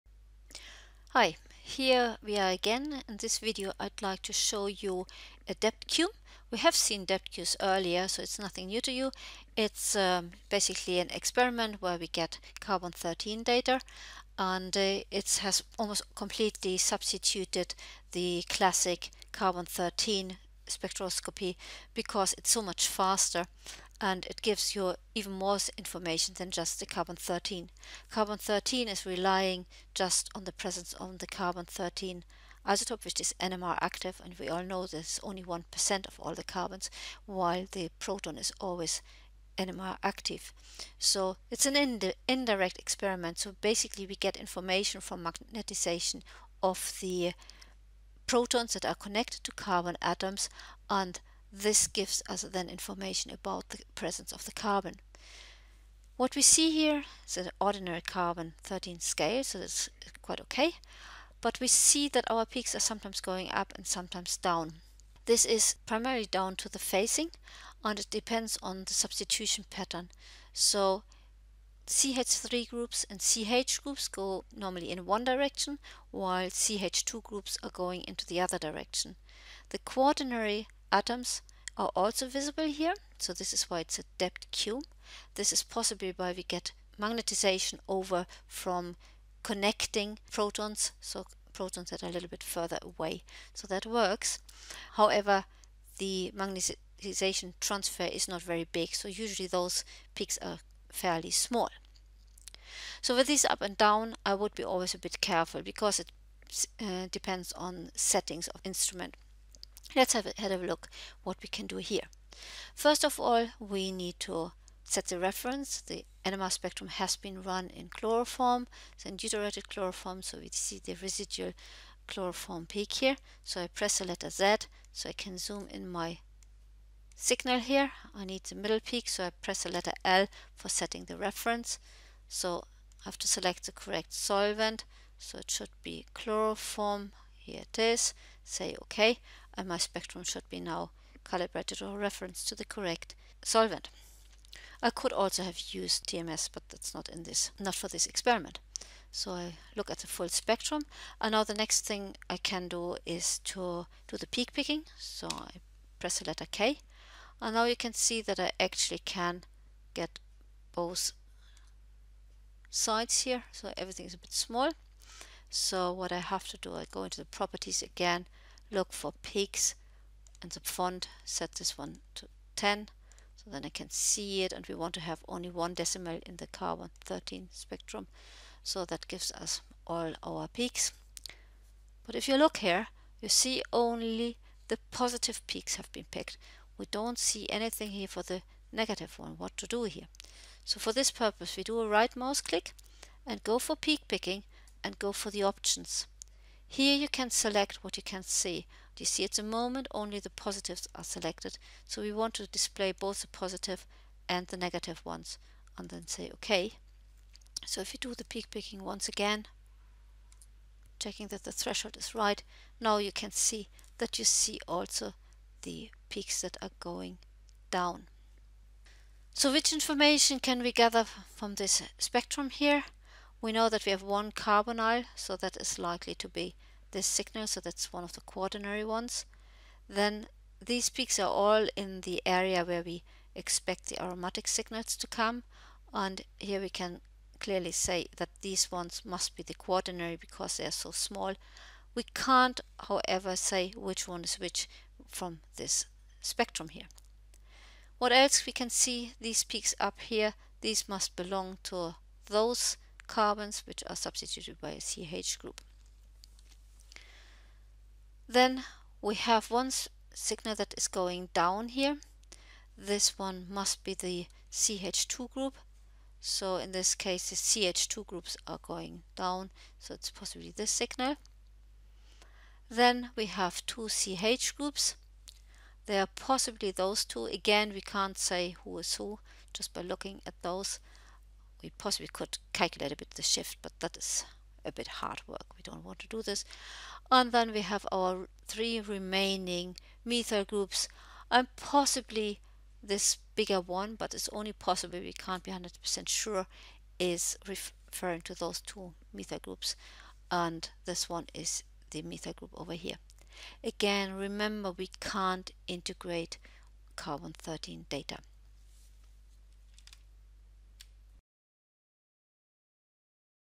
3 Ibuprofen DEPTq NMR
This video introduces how to process a DEPTq (and some analysis) using Ibuprofen as an example.
This video is directed at undergraduate chemistry students not at NMR specialists!